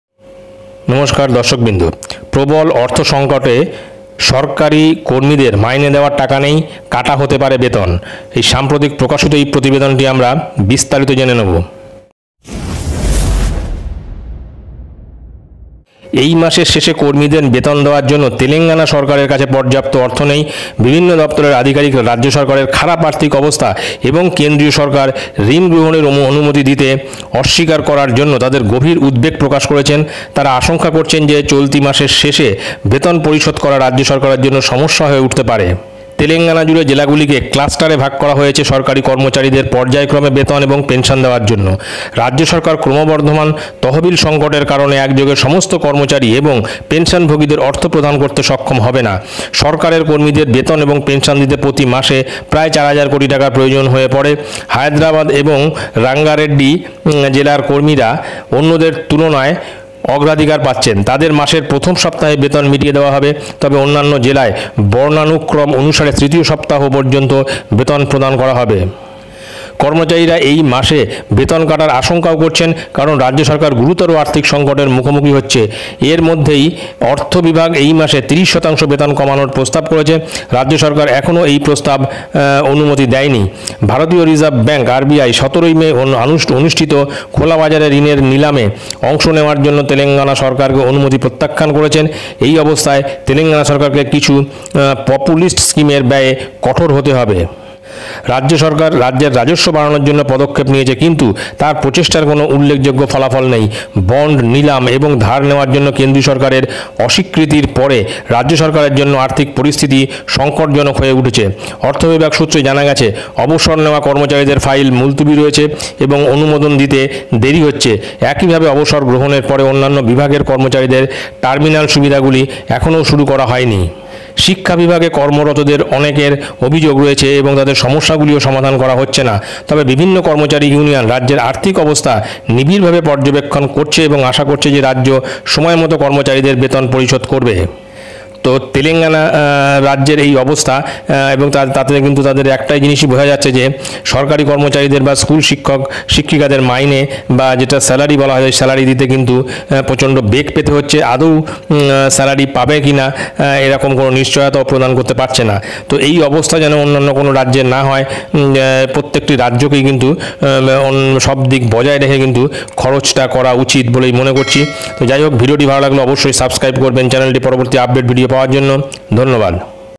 Big Breaking💥প্রবল অর্থ সঙ্কটে সরকারি কর্মীদের বেতনের টাকা নেই : কাটা হতে পারে বেতন💥বিস্তারিত জানুন
Big Breaking : প্রবল অর্থ সঙ্কটে সরকারি কর্মীদের বেতন দিতে পারবে না : কাটা হতে পারে বেতন।